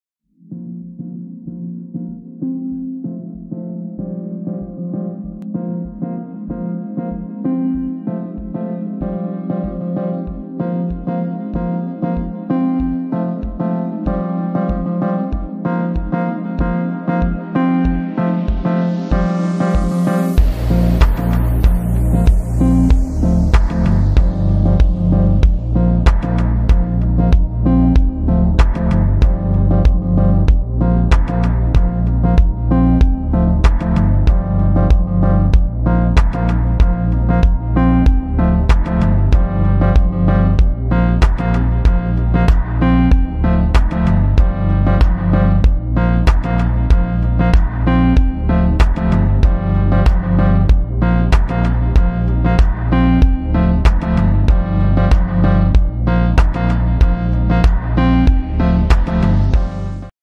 Knife Design in Solidworks
This is another timelapse video of a combat knife design by Solidworks and rendering the 3D model by Keyshot.